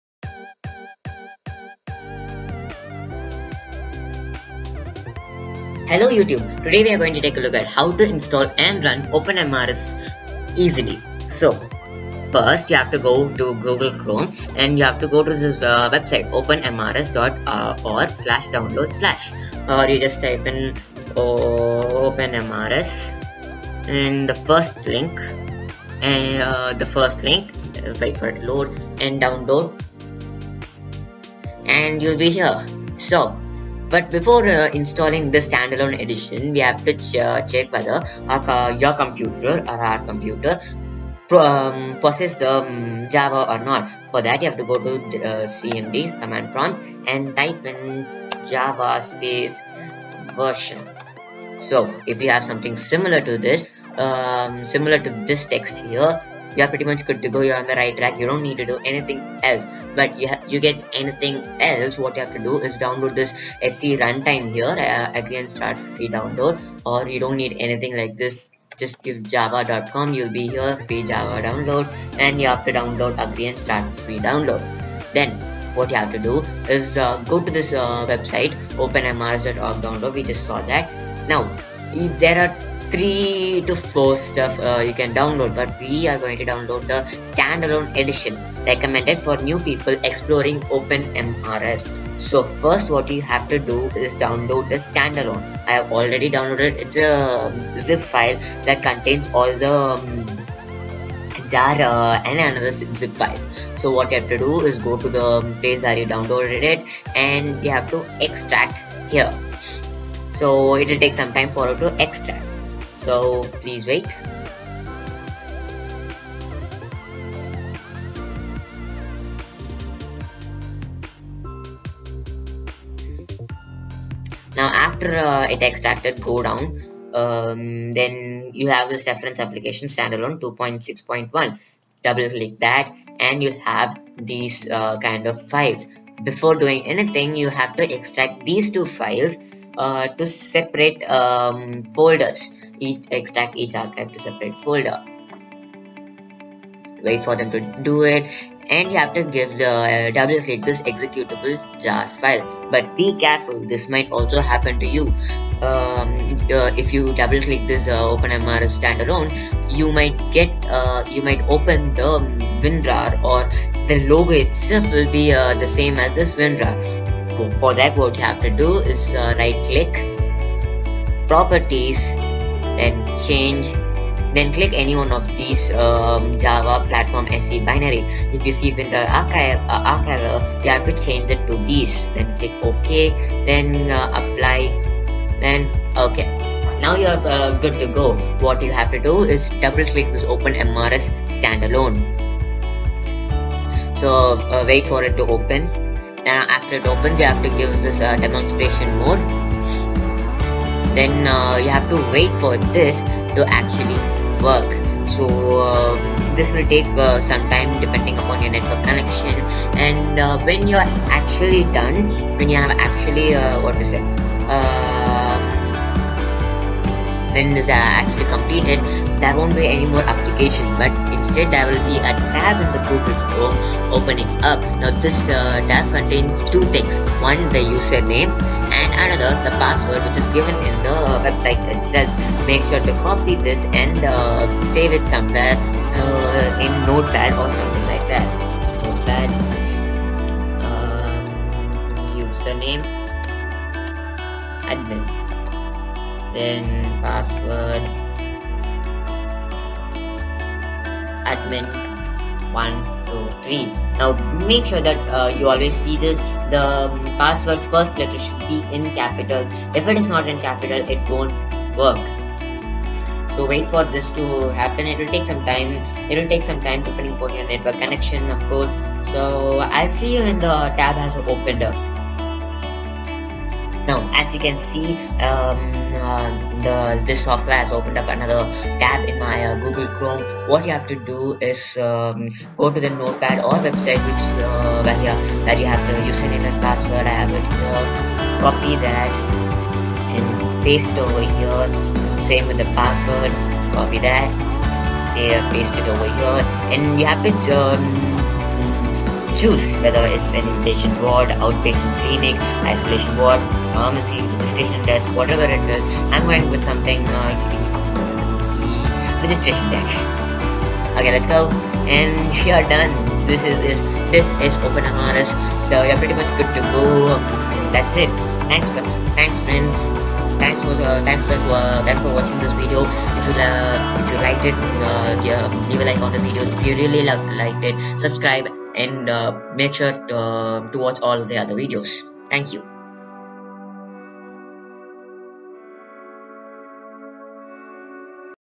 How to install OpenMrs easily!!!!!!
Hi youtube today Iam going to show you how to install and run Open Mrs easily without any effort.